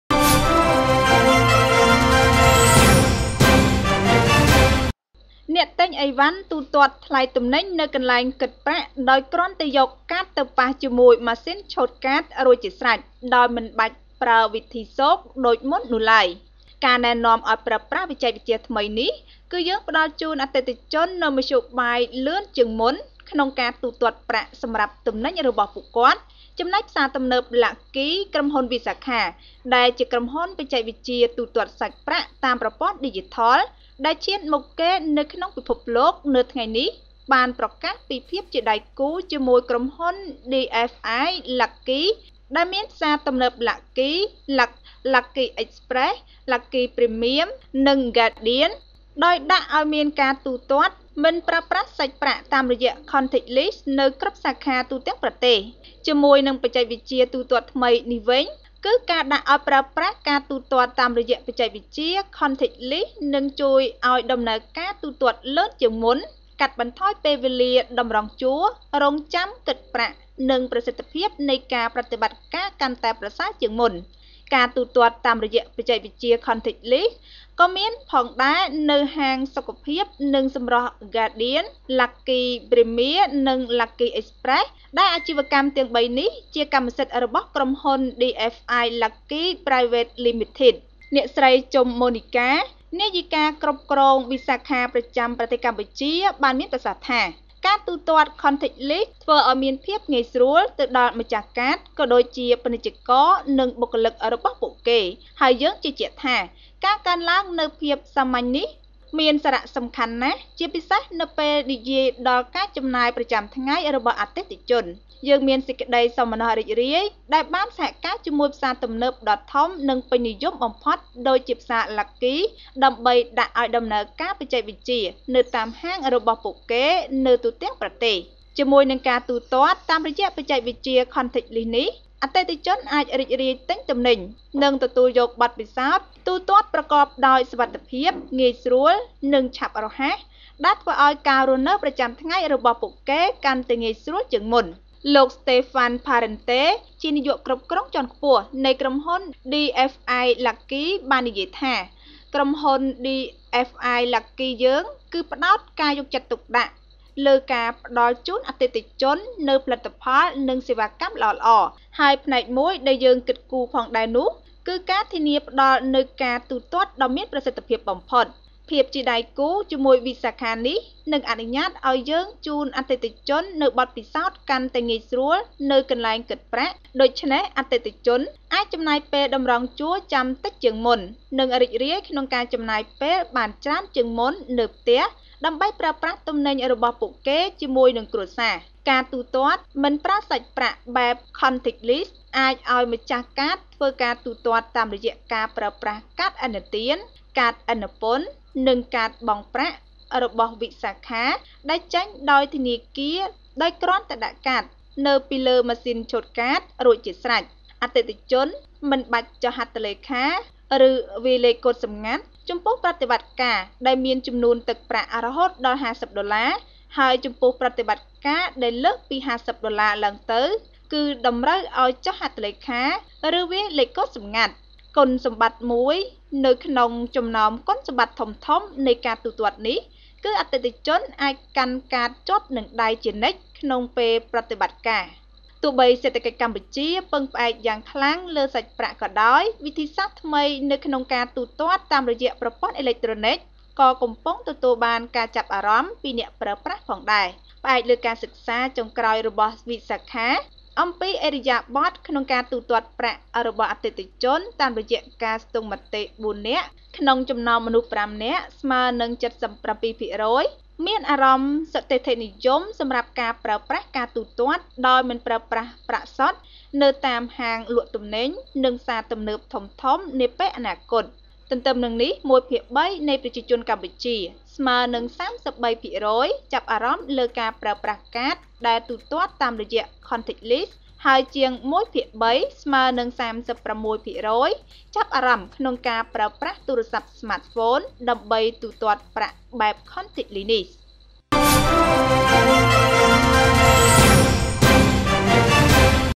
TVK New Release - Visa and DFI Lucky launch contactless payments at 41 branches across Cambodia
Visa, the world’s leader in digital payments, today announced a partnership with DFI Lucky Private Limited who operates Lucky Supermarket, Lucky Express, Lucky Premium and Guardian that will see contactless payments deployed at all branches.